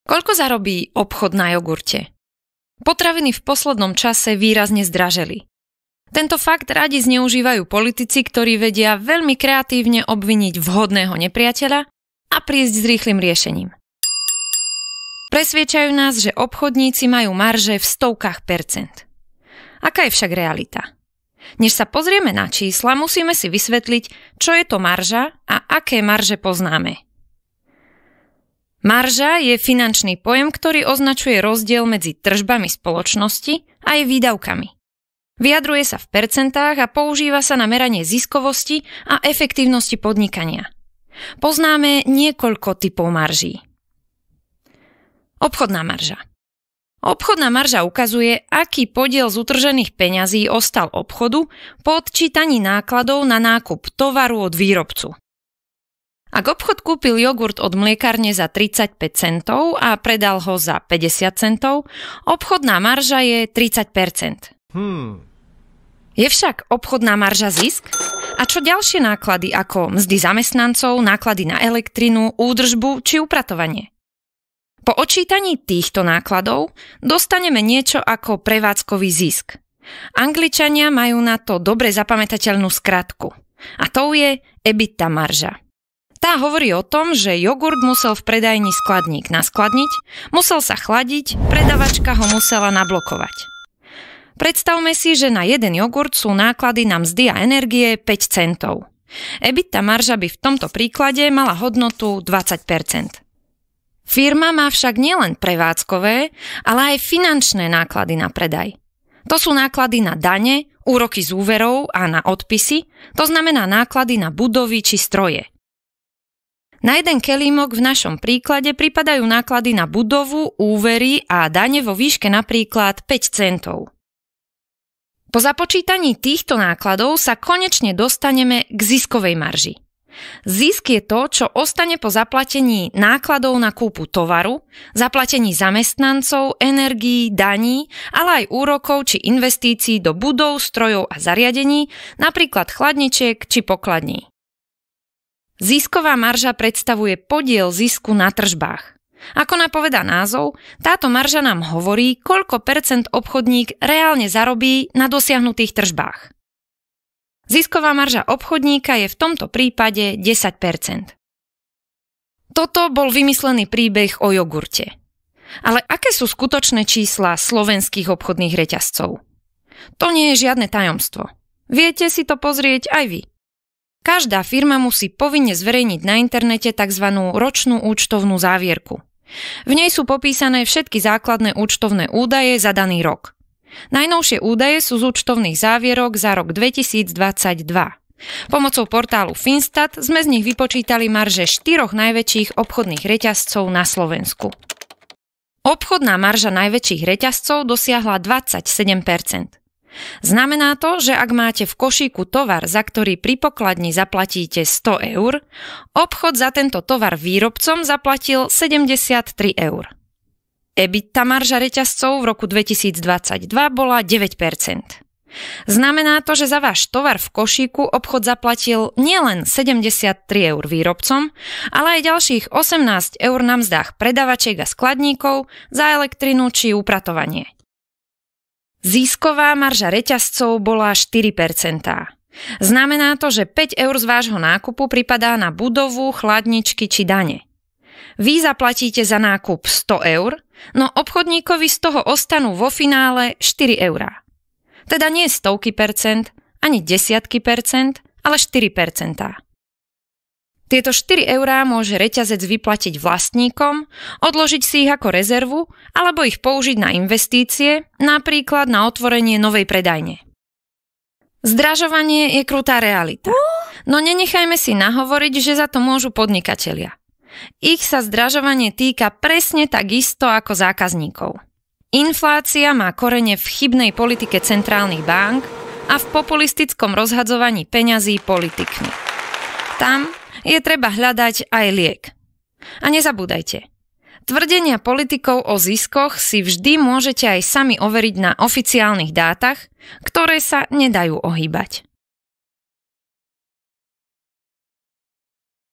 Koľko zarobí reťazec na jogurte?
Aké marže majú na Slovensku obchody s potravinami? A čo je to vôbec marža? Politici radi zvaľujú vinu za rastúce ceny na obchody. My sme sa preto pozreli na reálne čísla z účtovných závierok štyroch najväčších obchodných reťazcov.
Rastúcim cenám potravín sme venovali celú publikáciu. Zdarma si ju môžete stiahnuť tu